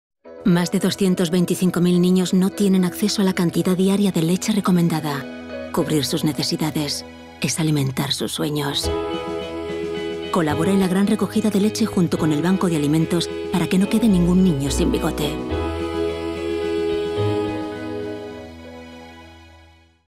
Campaña "Ningún Nilo Sin Bigote" 2019 de la Caixa_FESBAL
5ª edición de la campaña NingúnNiñoSinBigote de "recogida de leche" de la Caixa @FundlaCaixa y FESBAL @BancosDeAlimentos, que se realizará en las oficinas de la Caixa desde el 27 de mayo hasta el 14 de junio próximo. En paralelo, se pueden hacer donativos económicos en todas las oficinas de la Caixa y hasta el 31 de julio donaciones